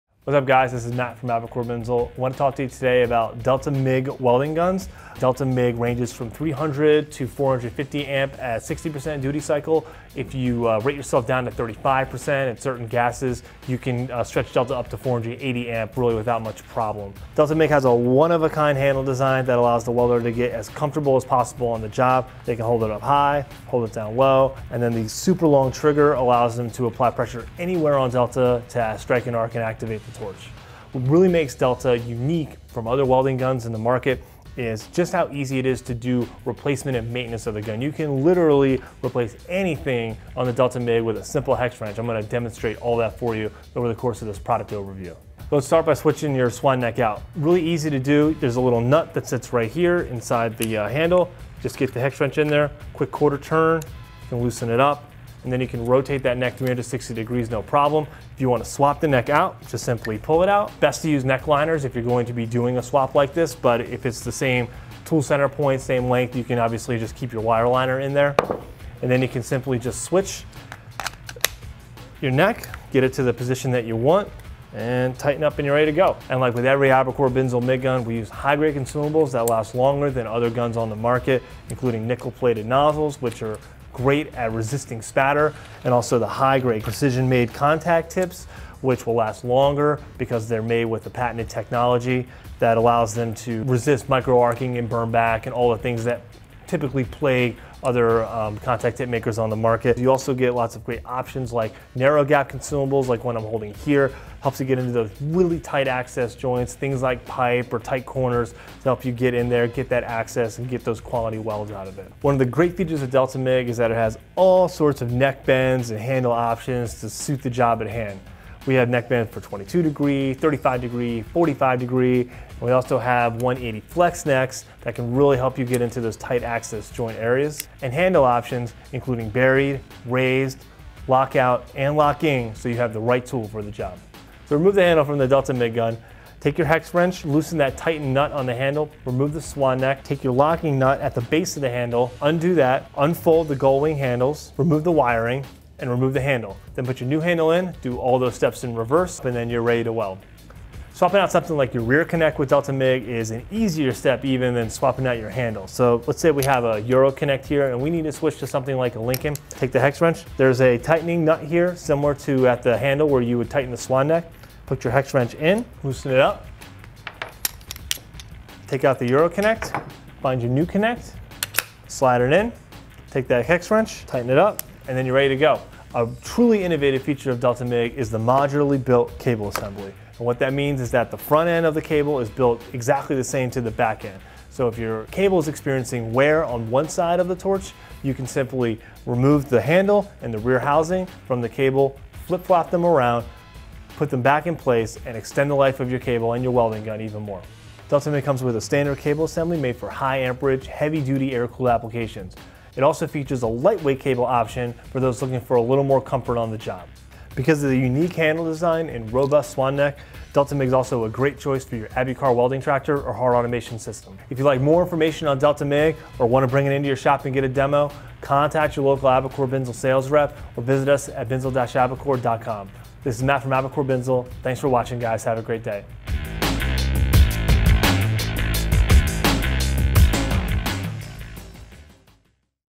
Delta MIG | Product Overview
A super extended trigger, modular design, tool-free maintenance, rotating necks, and flip-flop cable assembly make DELTA-MIG the most versatile, customizable, and easy to use heavy duty air-cooled welding gun in the industry.

Delta MIG ranges from 300 to 450 amp at 60% duty cycle. If you rate yourself down to 35% and certain gases, you can stretch Delta up to 480 amp really without much problem. Delta MIG has a one of a kind handle design that allows the welder to get as comfortable as possible on the job. They can hold it up high, hold it down low, and then the super long trigger allows them to apply pressure anywhere on Delta to strike an arc and activate the torch. What really makes Delta unique from other welding guns in the market is just how easy it is to do replacement and maintenance of the gun. You can literally replace anything on the Delta MIG with a simple hex wrench. I'm going to demonstrate all that for you over the course of this product overview. Let's start by switching your swan neck out. Really easy to do. There's a little nut that sits right here inside the handle. Just get the hex wrench in there. Quick quarter turn and loosen it up and then you can rotate that neck 360 degrees, no problem. If you you want to swap the neck out, just simply pull it out. Best to use neck liners if you're going to be doing a swap like this, but if it's the same tool center point, same length, you can obviously just keep your wire liner in there and then you can simply just switch your neck, get it to the position that you want and tighten up and you're ready to go. And like with every Abicor Binzel MIG gun, we use high-grade consumables that last longer than other guns on the market including nickel plated nozzles which are great at resisting spatter and also the high grade precision made contact tips which will last longer because there made with the patented technology that allows them to resist micro-arcing and burn-back and all the things that typically plague other contact in makers on the market. You also get lots of great options, like narrow gap consumables like what I'm holding here. Helps you get into those really tight access joints, things like pipe or tight corners, to help you get in there and get that access and get those quality welds out of it. One of the great features of Delta MIG is that it has all sorts of neck bands and handle options to suit the job at hand. We have neck bend for 22 degree, 35 degree, 45 degree. We also have 180 flex necks that can really help you get into those tight access joint areas and handle options including buried, raised, lockout and locking, so you have the right tool for the job. To remove the handle from the Delta MIG gun, take your hex wrench, loosen that tightened nut on the handle. Remove the swan neck, take your locking nut at the base of the handle, undo that, unfold the gull wing handles, remove the wiring and remove the handle. Then put your new handle in. Do all those steps in reverse and then you're ready to weld. Swapping out something like your rear connect with Delta MIG is an easier step even in swapping out your handles. So let's say we have a Euro connect here and we need to switch to something like a Lincoln. Take the hex wrench. There's a tightening nut here similar to at the handle where you would tighten the swan neck. Put your hex wrench in, loosen it up, take out the Euro connect, find your new connect, slide it in, take that hex wrench, tighten it up, and then you're ready to go. A truly innovative feature of Delta MIG is the modularly built cable assembly, and what that means is that the front end of the cable is built exactly the same to the back end. So if your cable is experiencing wear on one side of the torch, you can simply remove the handle and the rear housing from the cable, flip flop them around, put them back in place, and extend the life of your cable and your welding gun even more. Delta MIG comes with a standard cable assembly made for high amperage heavy duty air cooled applications. It also features a lightweight cable option for those looking for a little more comfort on the job. Because of the unique handle design and robust swan neck, Delta MIG's also a great choice for your ABI-CAR welding tractor or hard automation system. If you'd like more information on Delta MIG or want to bring it into your shop and get a demo, contact your local Abicor Binzel sales rep or visit us at binzel-abicor.com. This is Matt from Abicor Binzel. Have a great day.